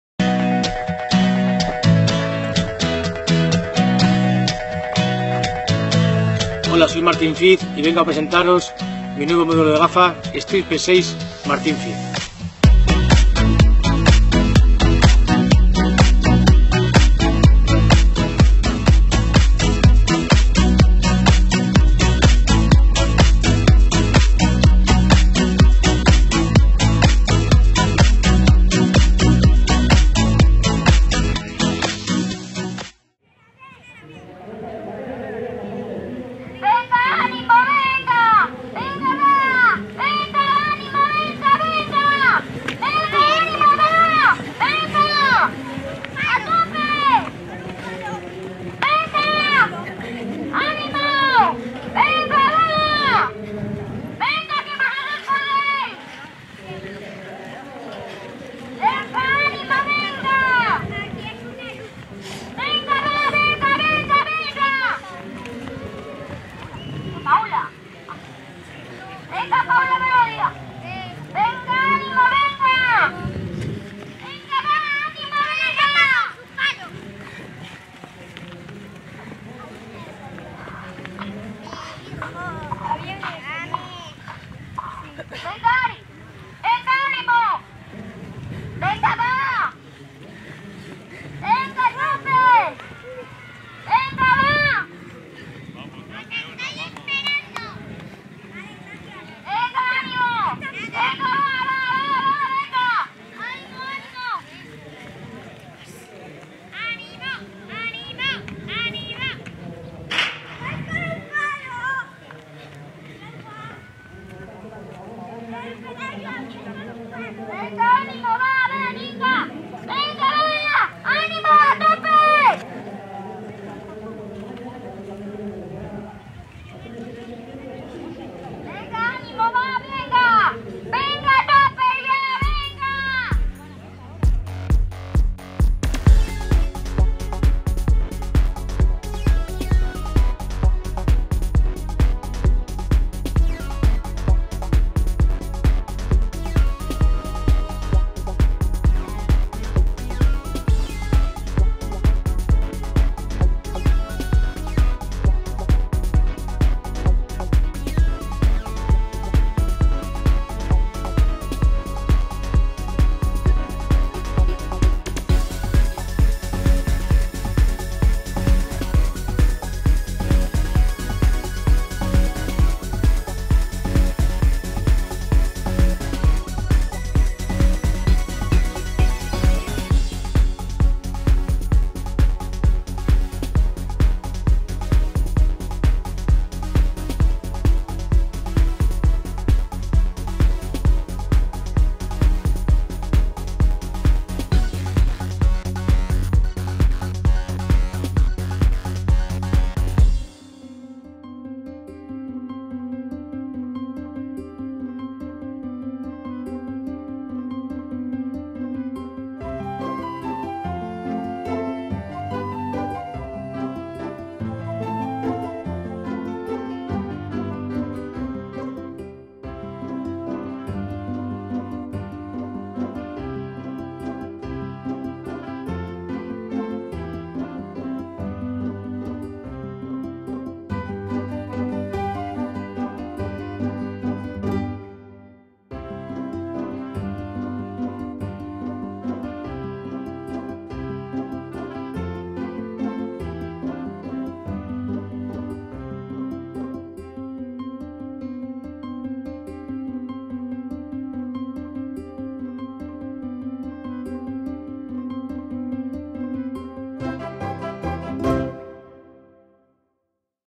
XIV CROSS ATAPUERCA 2017 - ÉLITE FEMENINA. Primera vuelta
Primera de las vueltas a su paso por el poblado, zona preciosa que caracteriza esta prueba internacional de campo a través de la prueba élite femenina del Cross de Atapuerca. 8 kilómetros en total completaron todas las atletas. Vídeo en cámara lenta al final.